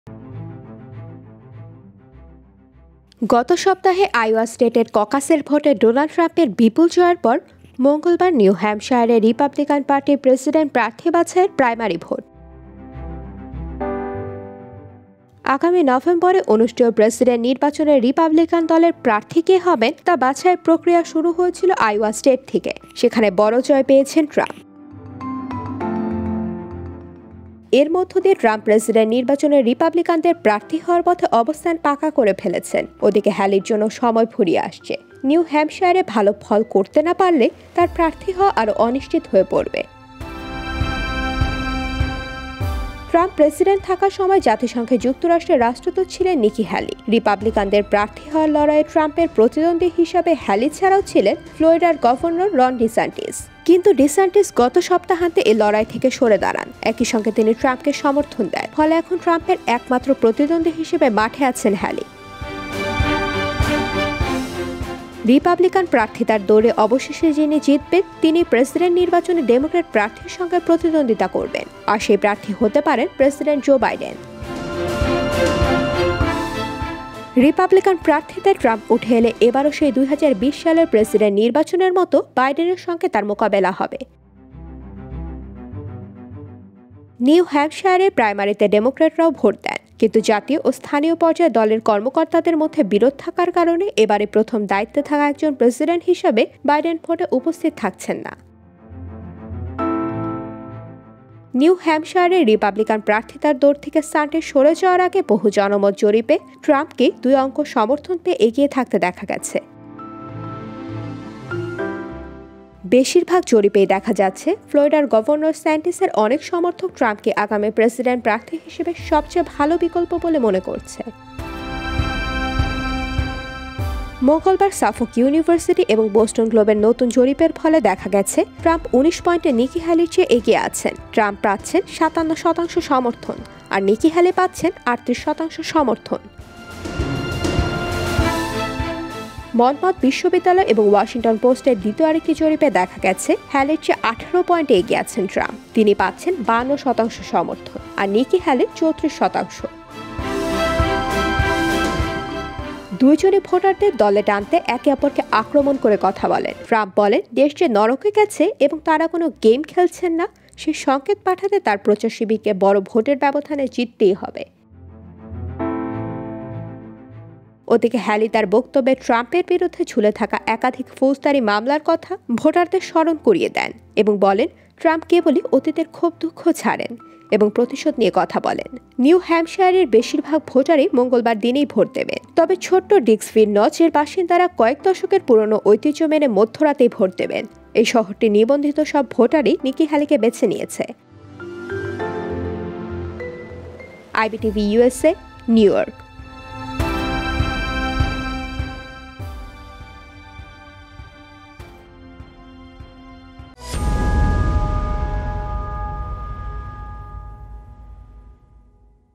এখন নিকি হ্যালিকে উড়িয়ে দিতে চান ট্রাম্প | Trump Election | USA
Now Trump wants to fire Nikki Haley